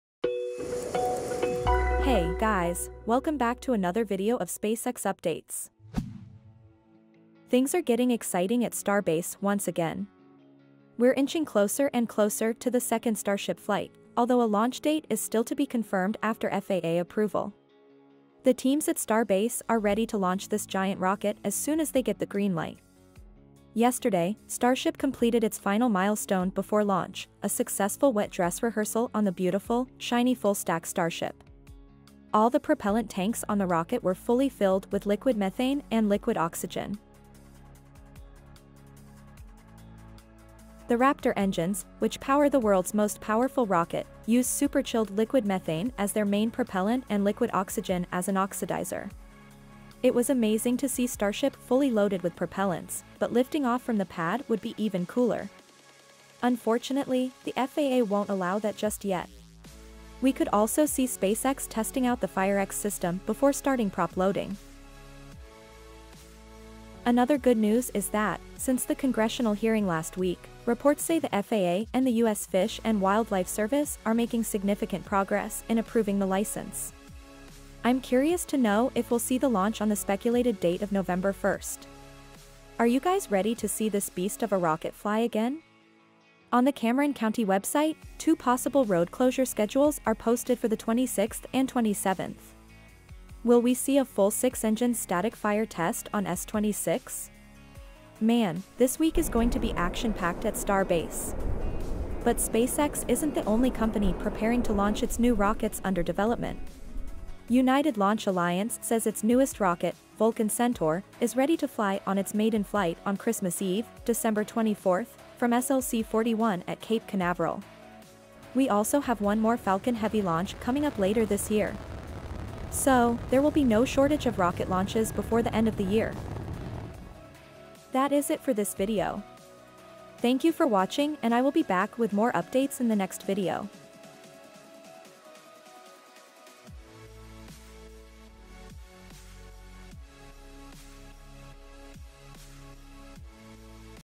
Starship is Super Ready for Flight | Progress on the FAA License | SpaceX Updates
SpaceX completed the last milestone ahead of its second Starship Launch in the coming weeks.
A Wet Dress Rehearsal test was conducted simulating a flight flight rehearsal.

Credits and Links to the footage I used in my videos with permission: